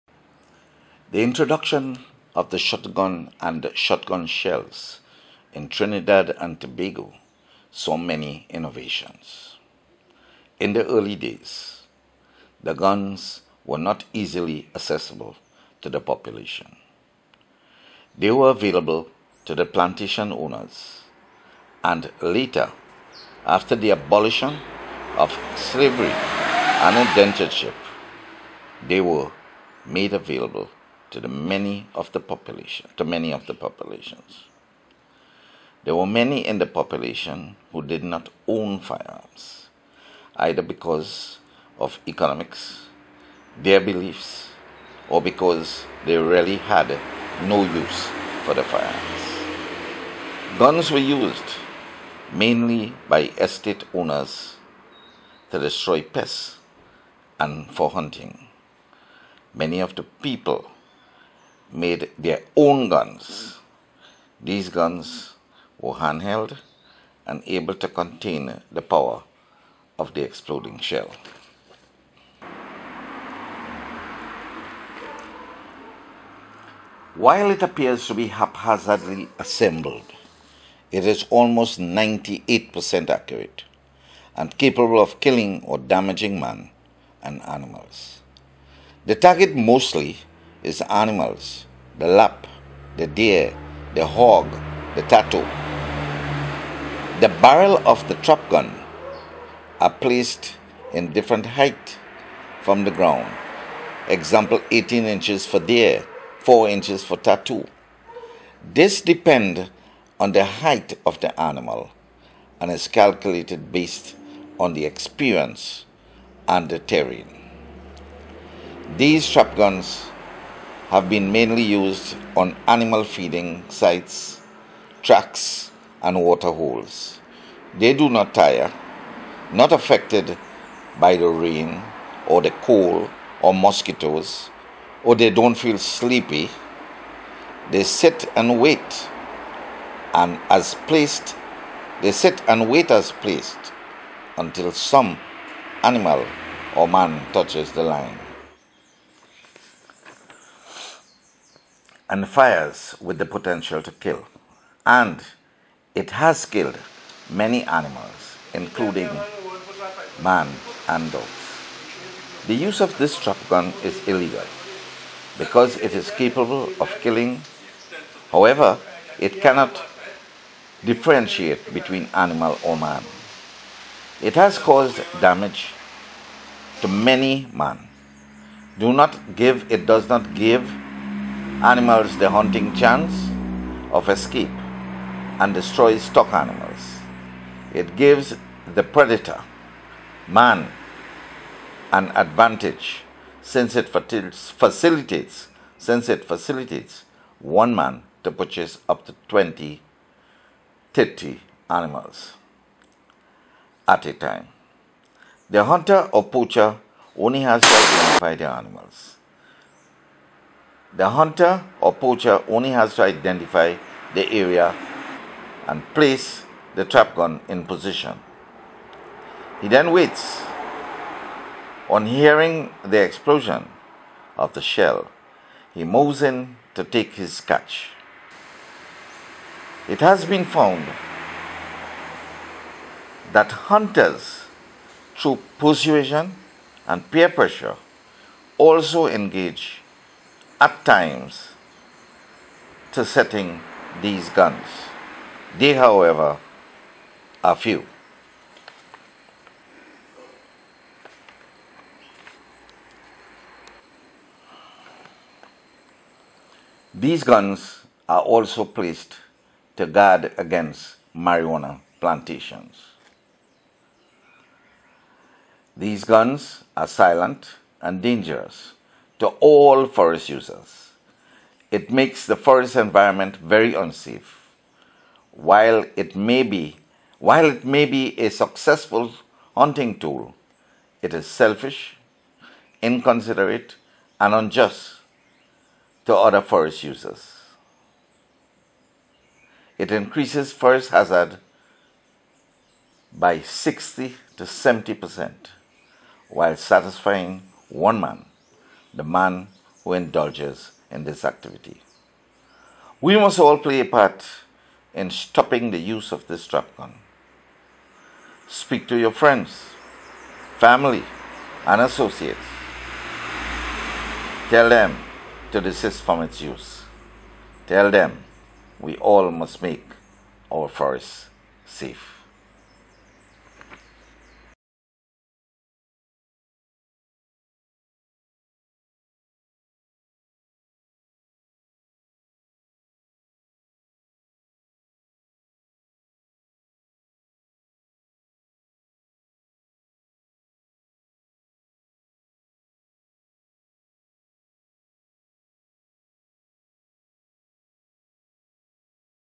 Trap guns invented and used in Trinidad & Tobago
Trap Guns invented and used in Trinidad & Tobago. It is used for hunting and protection  of Marijuana Plantations